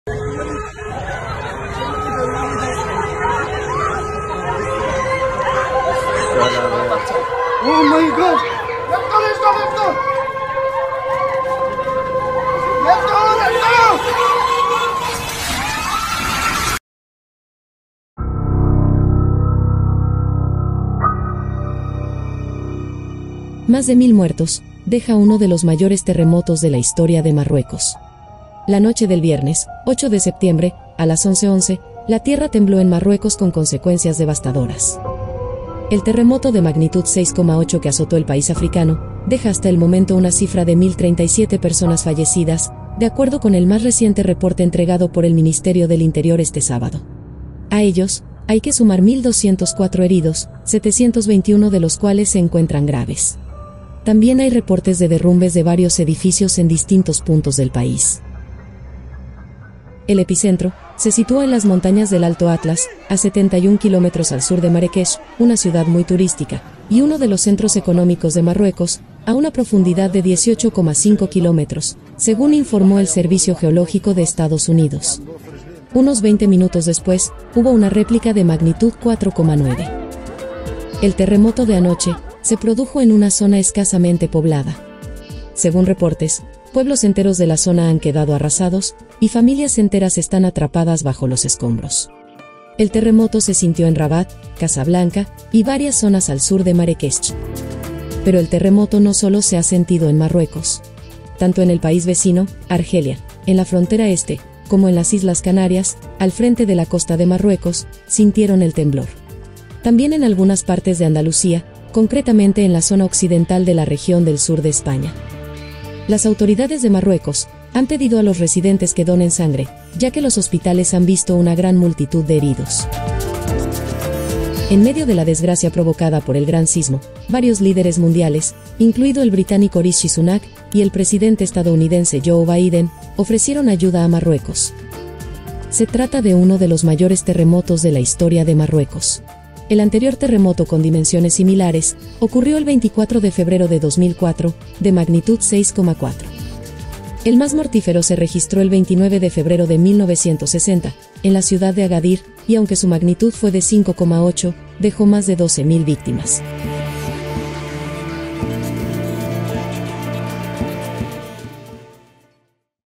Revelan IMPACTANTE VIDEO del Terremoto En Marruecos Que Dejó Más De 1.000 Muertos El 8 De Septiembre
Revelan IMPACTANTE VIDEO del Terremoto En Marruecos Que Dejó Más De 1000 Muertos El 8 De Septiembre